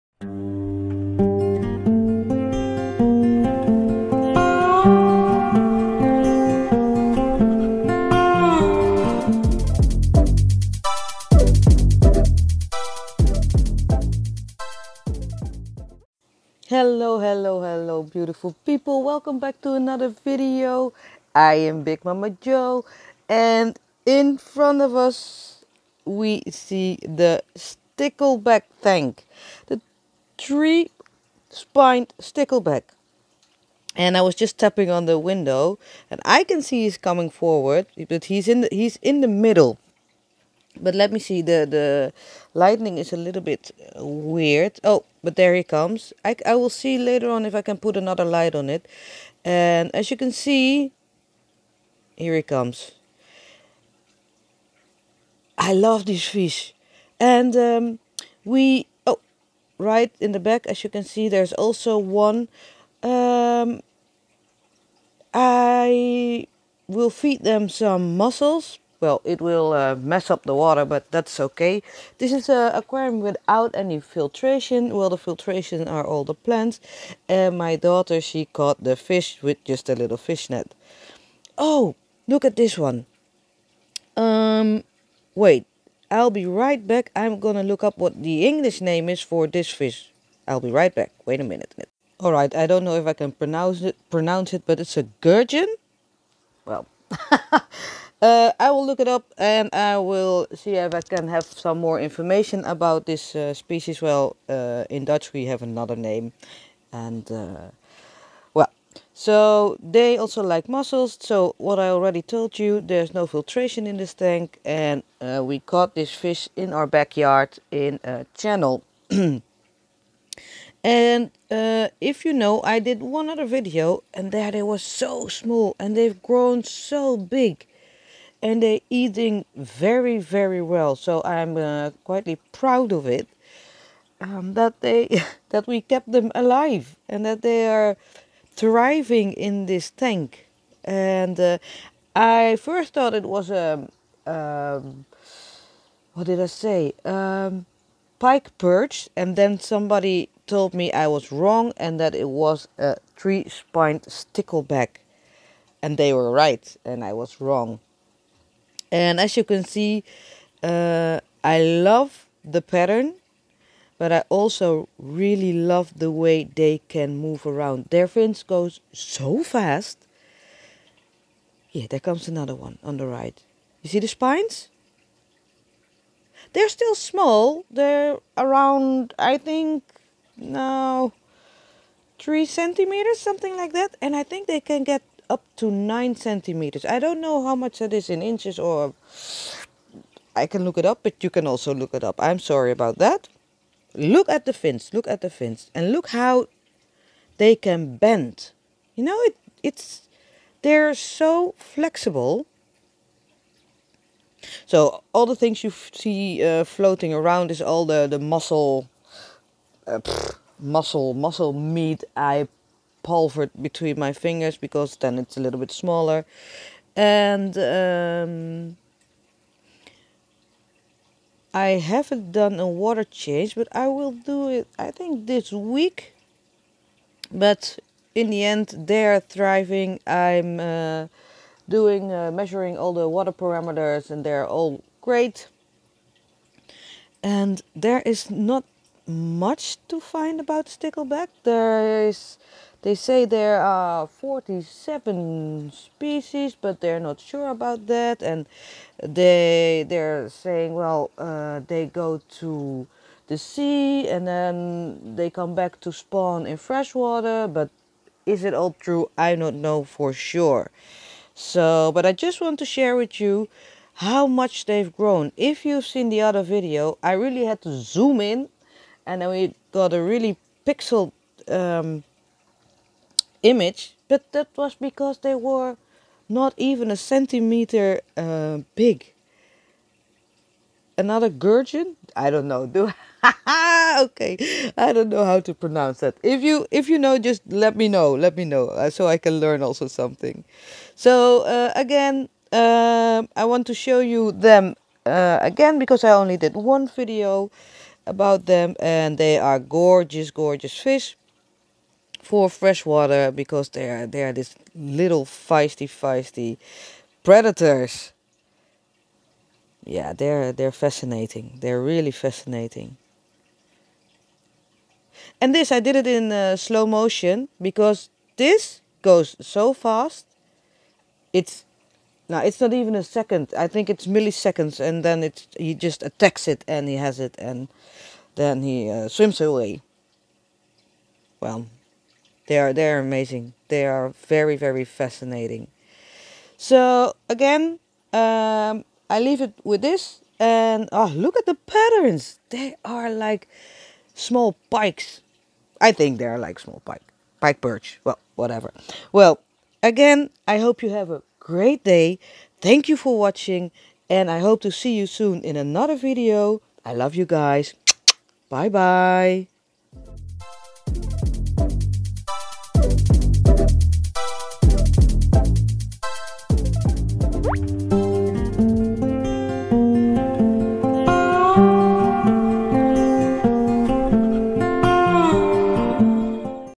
🇺🇸 update about the Threespine Stickleback - special fish trying to grow out
today I want to take you back to the threespine stickleback we caught in our backyard ditch. they were so small, under a cm big. now they're about 3cm and growing fast.